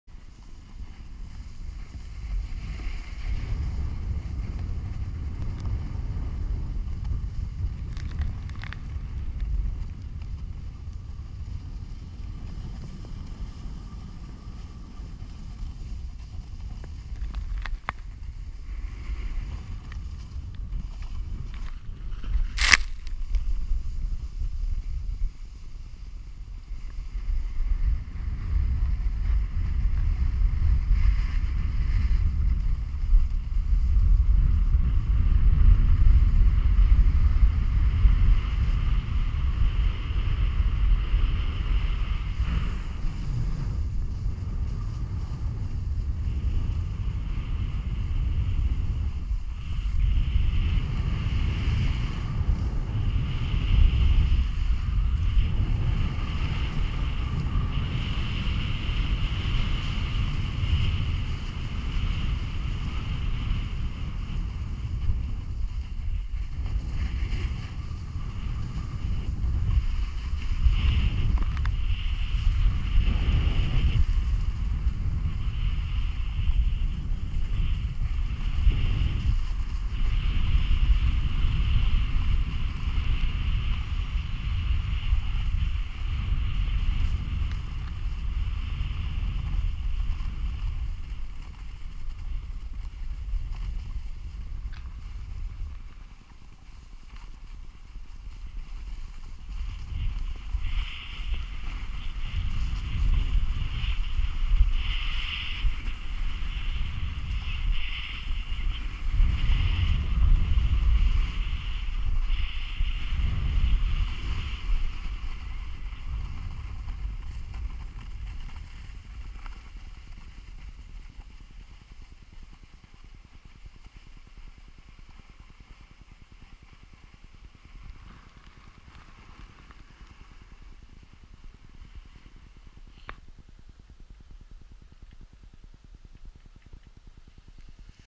Trace: Skiing - Matt Sullivan at Boreal
Trace auto edited Matt Sullivan's skiing session at Boreal on Jan 03, 2016.

Runs: 7
Min of Skiing: 17
Vertical M: 2.6k ft
Distance: 3.5 mi
Calories Burned: 572
Max Speed: 39.3 mph
Max sustained speed: 32.0